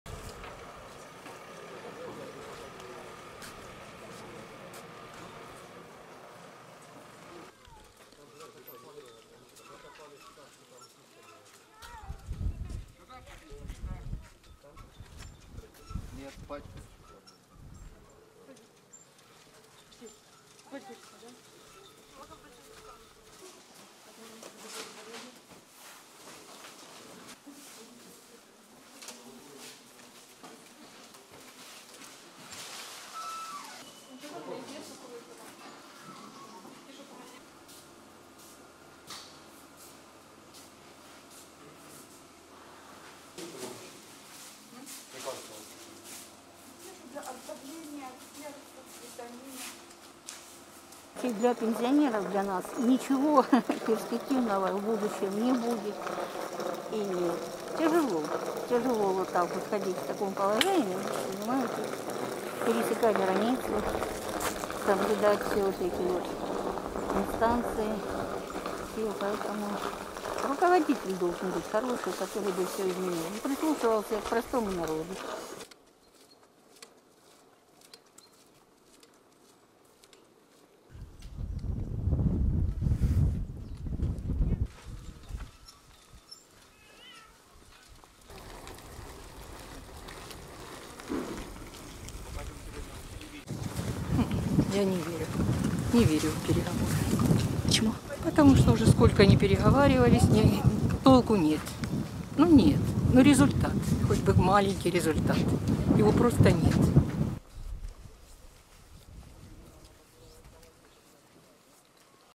Some in Luhansk region skeptical about talks
(11 Feb 2022) Ukrainian residents in the government-controlled part of Luhansk region in eastern Ukraine say they are skeptical about peace talks currently underway in Berlin attempting to reduce tensions between Ukraine and Russia.
Thurday's talks in Berlin between Ukraine and Russia, joined by France and Germany, at first held hope for some progress as they lasted into Friday morning, but at the end, nothing palpable emerged, with a Russian senior official noting no "significant results." It remained unclear when and how the next attempt at a breakthrough would be made.
The lack of visible results at the talks demotivated residents near the line of contact between Russia-backed seperatists and Ukrainian government forces, who say they see little hope that a breakthrough could be reached.
"For us pensioners, it does not look like anything will get better in the future," said Svitlana, 65, who had just crossed the line of contact.
"We need a good leader who would change everything and listen to regular people," she added.
After the talks, Russian Deputy Chief of Staff Dmitry Kozak said Ukraine had firmly refused to commit to a dialogue with the rebels on a political settlement, blocky any further movement.
Ukrainian envoy Andriy Yermak sounded a more positive note, noting that the parties had agreed to continue their discussions and hailed the four-way talks as an "effective and efficient platform."
The Berlin talks are part of renewed diplomatic efforts to resolve the biggest security crisis between Russia and the West since the Cold War.
A 2015 peace deal brokered by France and Germany in a similar so-called Normandy format helped halt large-scale battles in the wake of the 2014 Russian invasion, but regular skirmishes have continued. The Kremlin has accused Kyiv of sabotaging the agreement, and Ukrainian officials argued in recent weeks that implementing it would hurt their country.